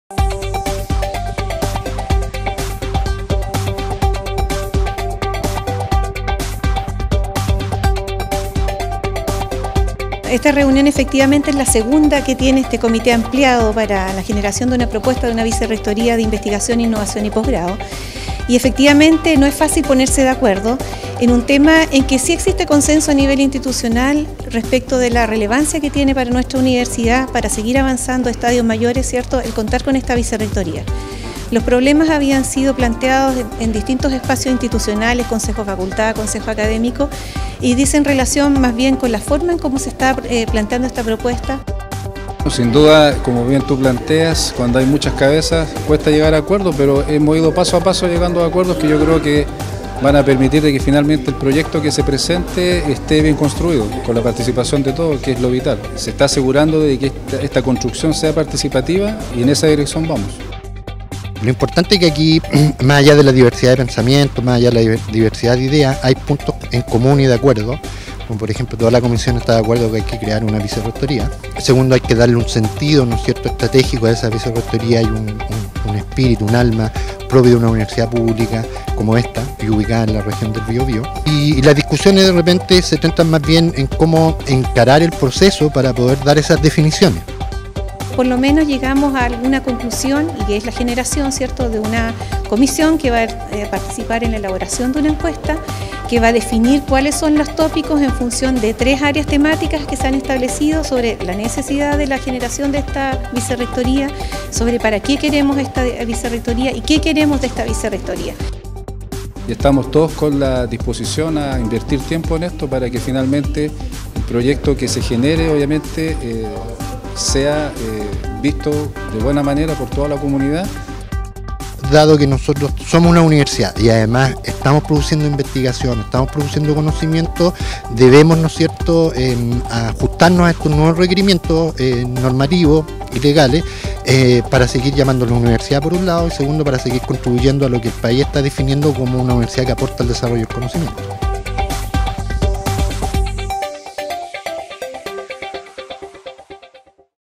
Comité Ampliado Comprometido con la Nueva Institucionalidad y Postgrado
En Chillán, este martes 9 de agosto continuaron las reuniones de trabajo del Comité Ampliado, esta vez para ver temas de presentación, estructura y elaboración de encuesta que permitan la más amplia participación de la comunidad universitaria en la creación de la nueva institucionalidad de Investigación, Innovación y Postgrado (VRIIP) de la Universidad del Bío-Bío.

En la oportunidad se presentó una propuesta de trabajo del subcomité encargado de elaborar y desarrollar herramientas que permitan conocer la opinión de la comunidad universitaria en torno a esta nueva institucionalidad. Entre las metodologías y herramientas participativas se señalan la realización de un Focus Group y una encuesta On Line, que responda a las preguntas de por qué, para qué se quiere esta Vicerrectoría.